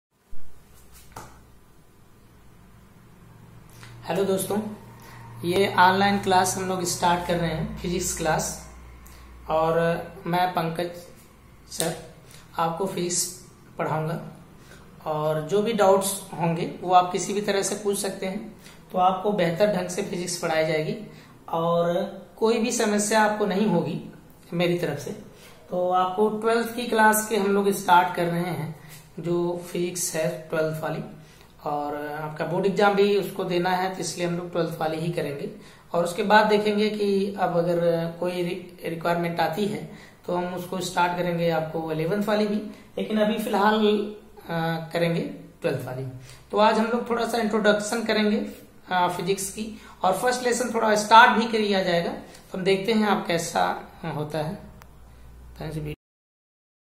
Introduction of YouTube classes
physics class 12th taking about YouTube classes  Topics  interoduction YouTube classes  lecture by Pankaj sir Language English mixed with hindi